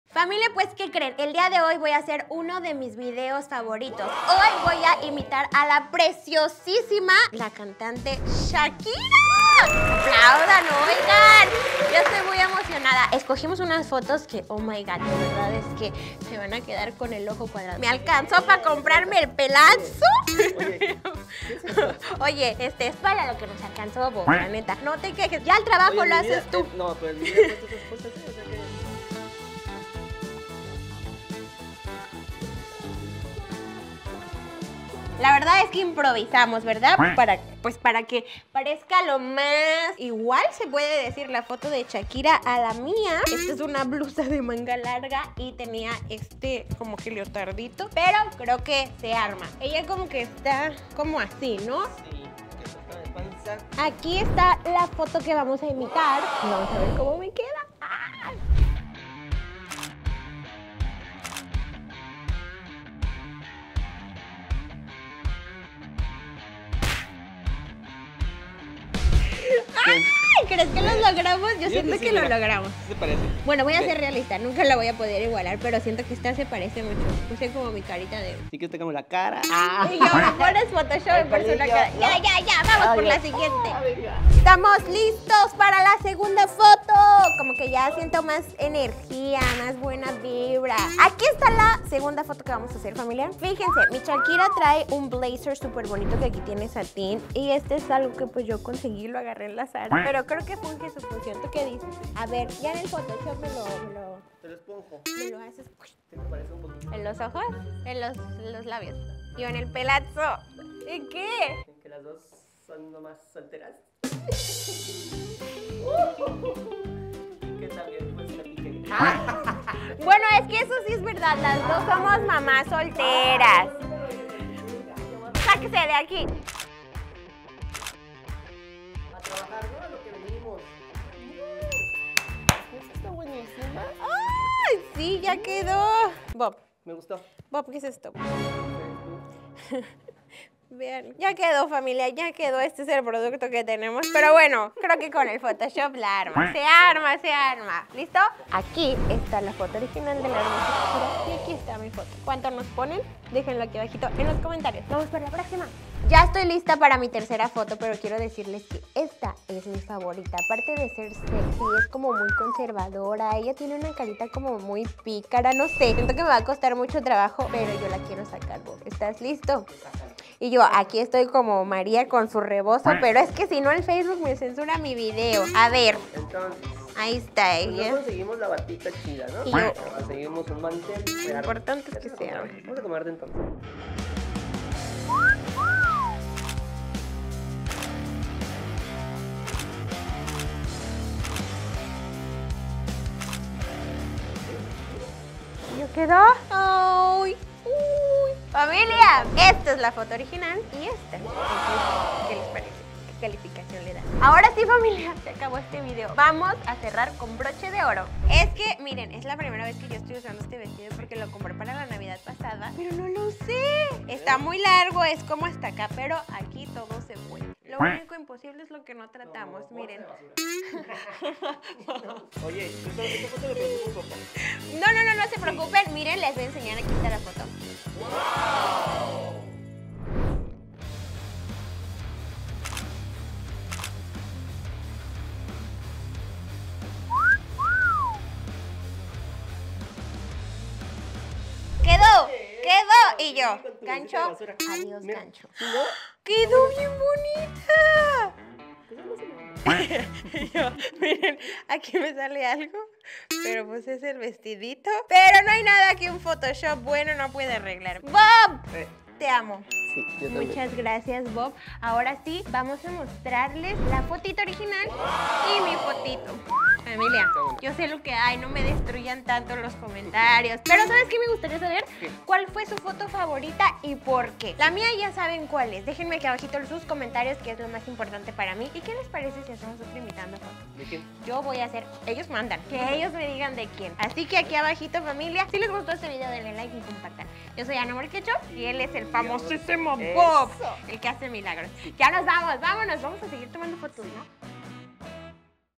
Imitando fotos de Shakira. Las dos son mamás solteras
Familia el día de hoy voy a hacer uno de mis videos favoritos, imitando fotos de Shakira y la verdad van a ser unas fotitos muy hermosas. Déjenme saber en los comentarios que tan similares me salieron.